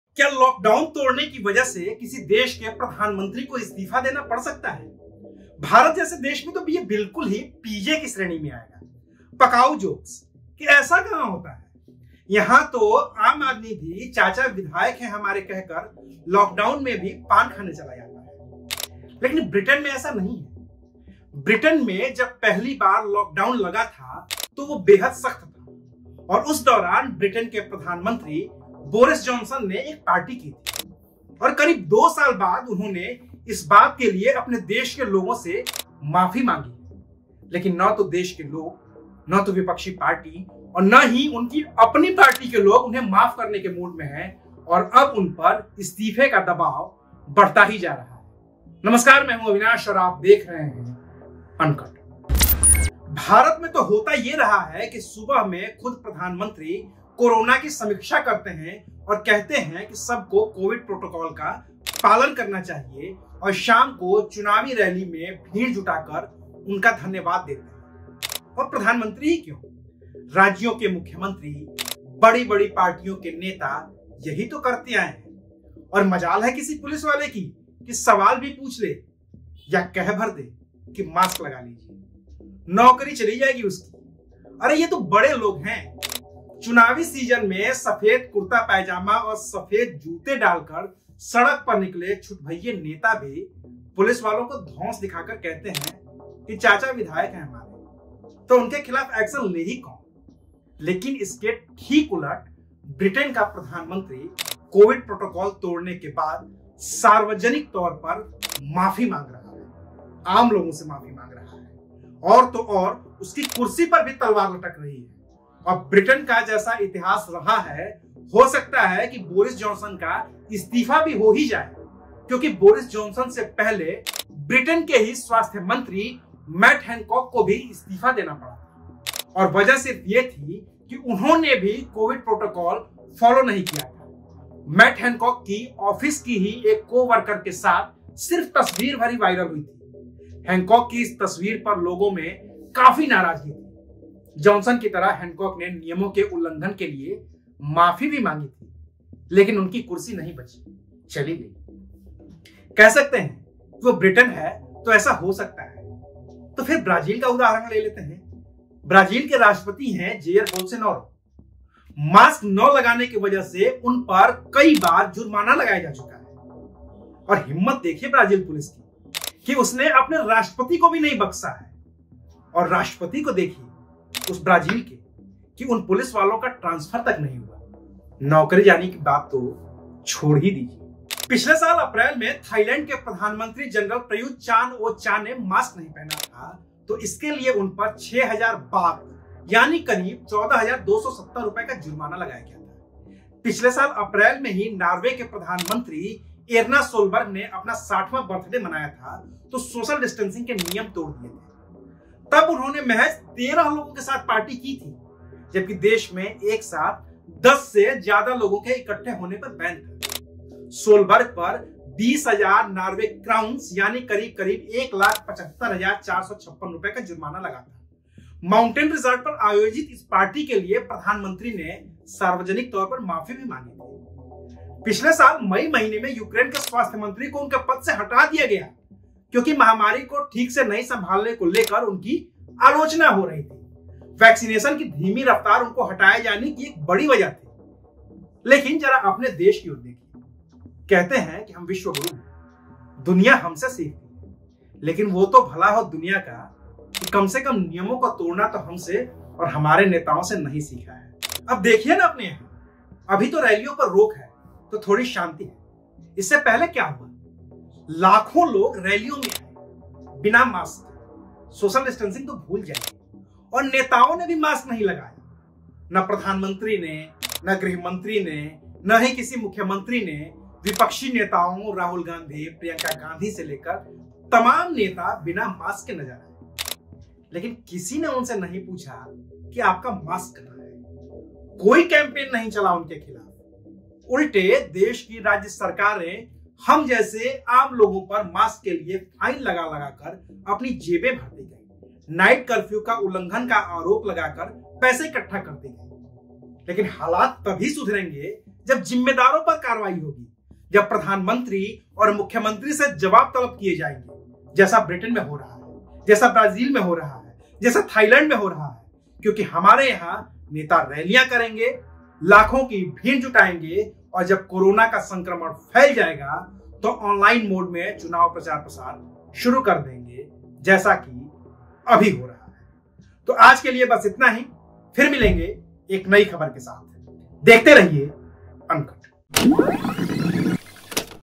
Covid 19 Protocol तोड़ने पर President से PM तक नप गए, India में Politicians पर क्यों नहीं होता ऐक्शन
क्या लॉकडाउन (Lockdown) तोड़ने की वजह से किसी देश के प्रधानमंत्री पर इस्तीफे का दबाव आ सकता है. भारत में तो कोई टोकने की भी हिम्मत नहीं करता. लेकिन ब्रिटेन में ऐसा नहीं है. ब्रिटेन में जब पहली बार लॉकडाउन लगा था तो उस दौरान ब्रिटेन के राष्ट्रपति बोरिस जॉनसन (Boris Johnson) ने पार्टी की थी. और अब जब लोगों को पता चला तो उन्होंने माफी मांग ली है, लेकिन उनपर इस्तीफे का दबाव बढ़ रहा है. ब्राजीले के राष्ट्रपति बोलसेनारो (Jair Bolsonaro) पर मास्क न पहनने के लिए जुर्माना लगा, थाईलैंड के प्रधानमंत्री जनरल प्रयुत चान-ओ-चा (Prayut Chan-o-cha) ने मास्क नहीं पहना तो उनपर जुर्माना लगा, नॉर्वे की प्रधानमंत्री एर्ना सोलबर्ग (Erna Solberg)  ने बर्थडे मनाने के लिए सोशल डिस्टेंसिंग के नियम तोड़े तो उनपर भी जुर्माना लगा, लेकिन भारत में ऐसा सपने में भी नहीं सोचा जा सकता है. देखिए अविनाश राय  (Avinash Rai) की ये रिपोर्ट.